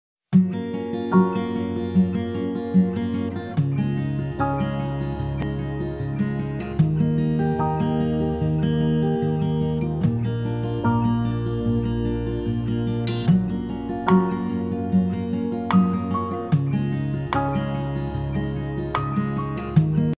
Filtration with Pipeless filter/Membrane swimming pool Bluewave pools india
Hi guys. This is Pipeless filter swimming pool. Without any extra wall breaking inlets and plumbing.